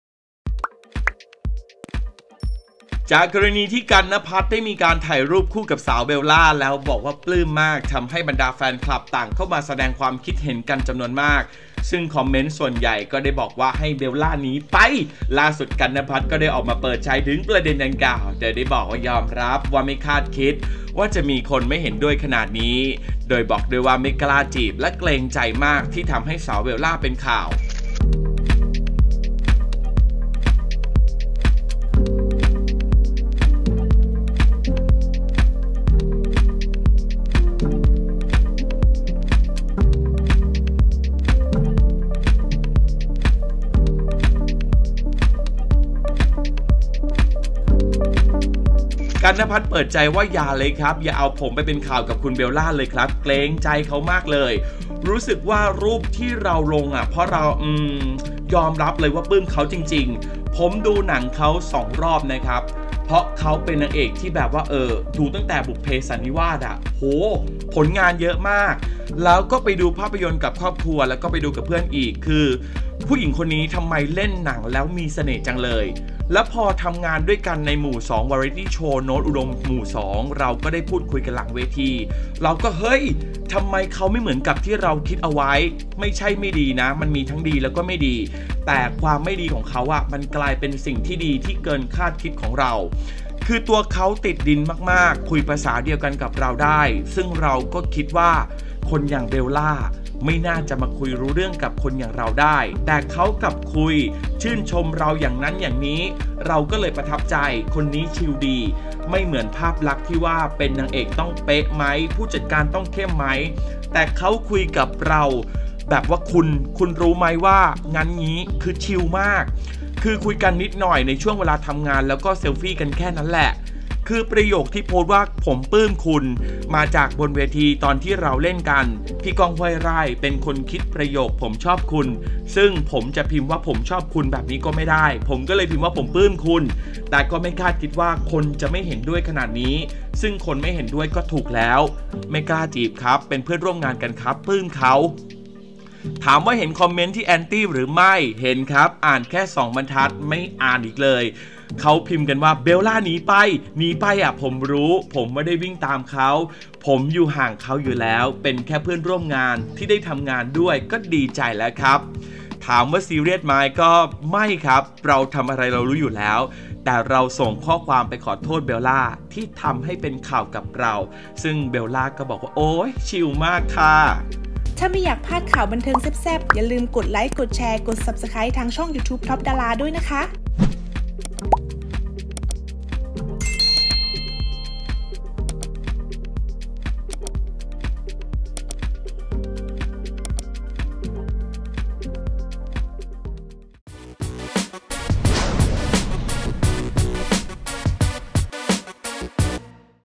สงสาร "กัน นภัทร" หลังเปิดใจ ปลื้มจริงแต่ไม่กล้าจีบ "เบลล่า ราณี" | TOP DARA
จากกรณีที่"กัน นภัทร" ได้ถ่ายรูปคู่สาวเบลล่านั้น แล้วบอกปลื้มมาก ทำให้แฟนคลับต่างเข้ามาแสดงความคิดเห็นกันเป็นจำนวนบ้าง ซึ่งคอมเมนต์ส่วนใหญ่ก็ได้บอกว่า "ให้หนีไป" ล่าสุดเจ้าตัวก็ได้ออกมาเปิดใจถึงประเด็นดังกล่าวแล้ว โดยได้บอกว่า "ยอมรับไม่คาดคิดว่า คนจะไม่เห็นด้วยขนาดนี้ ลั่นไม่กล้าจีบและเกรงใจที่ทำให้เป็นข่าวด้วย"

ติดตามความเคลื่อนไหวจาก TOP DARA ได้อีกช่องทาง FB TOP DARA